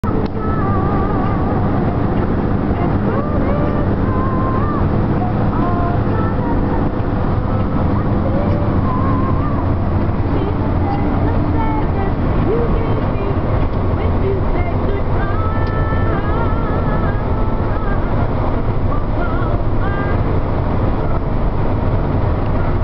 G-Force S2000!
Me in my dirty black S2000 in France this year!  The van couldn't keep up!  Adam